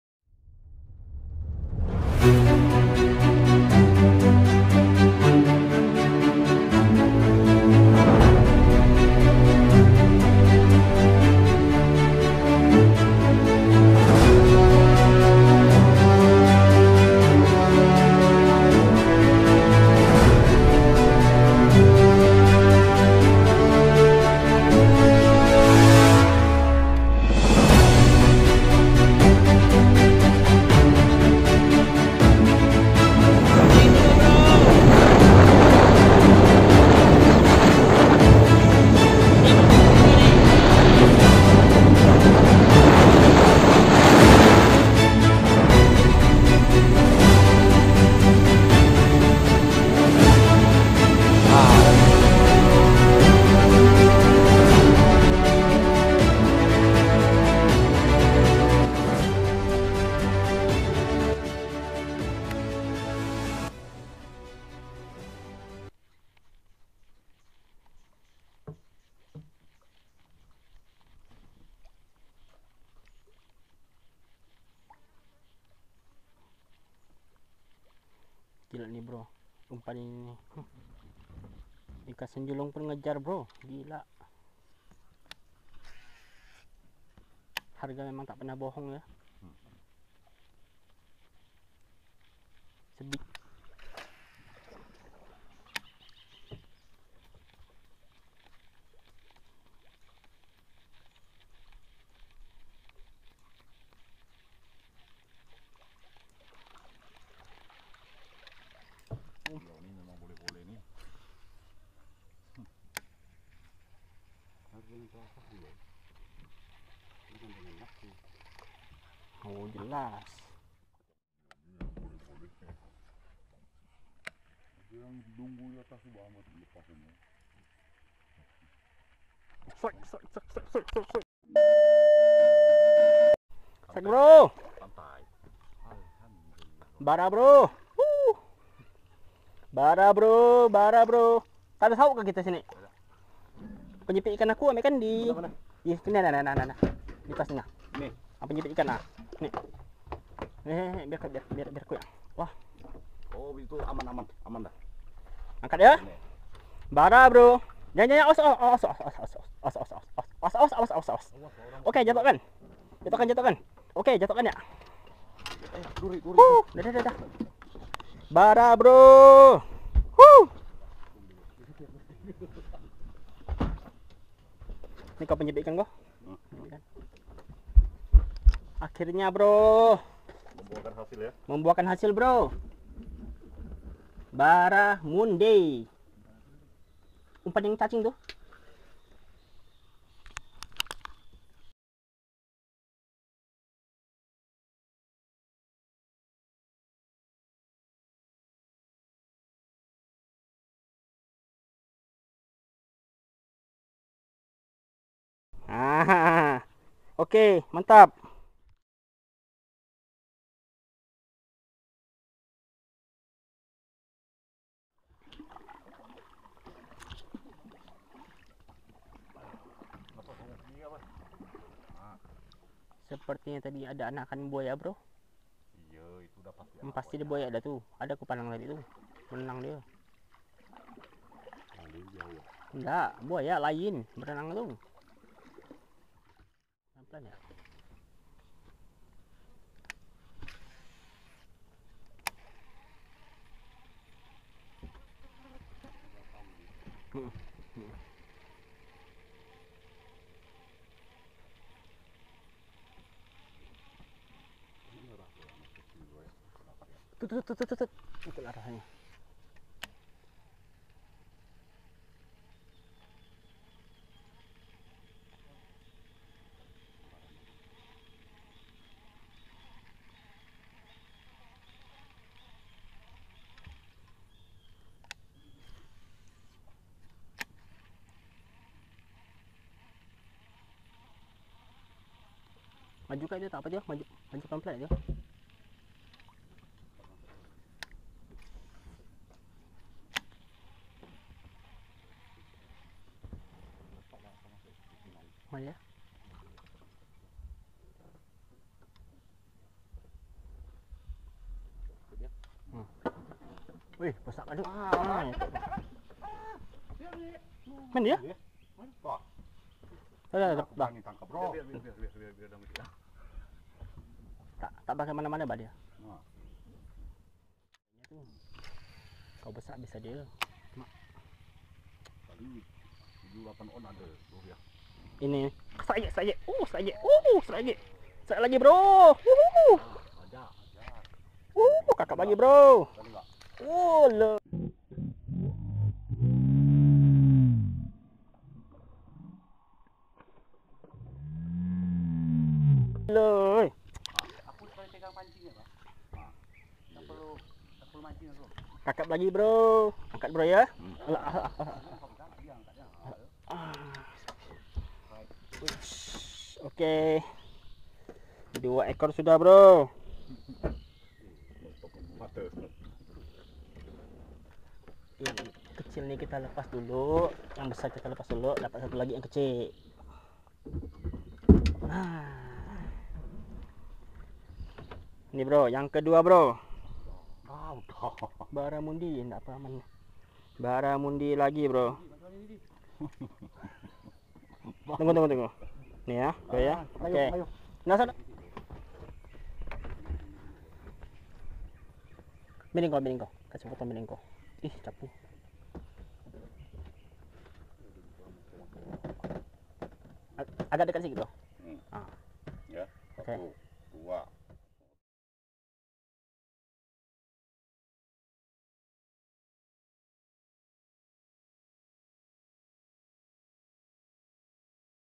Perburuan Barramundi Air Tawar Pulau Maya
Perburuan Barramundi Air tawar - air asin di Area Pulau Maya, Kayong utara, kalimantan barat.

Spot yang belum pernah disentuh oleh Caster, suasana yang alami dan sejuk..

tackle info :

Rod : Shimano Zodias 1610MH-2 25lb
Reel : Shimano Bantam MGL XG
Line : Shimano Kairiki 8  30lb
Leader : Relix Samar 40lb